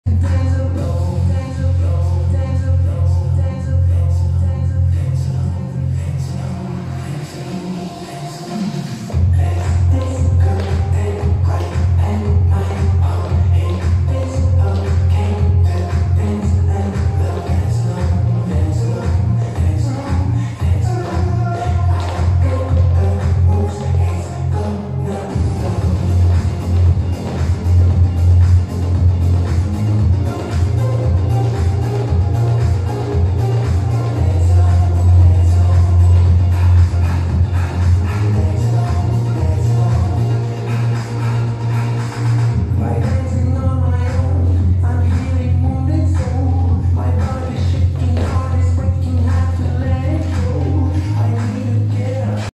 LithuaniaGoesYellow LINEDANCE
Neringos Kirklienės šokių mokykla "LINEDANCE"
Linijinių šokių Geltonoji banga už "THE ROOP" sėkmęs Eurovizijoje!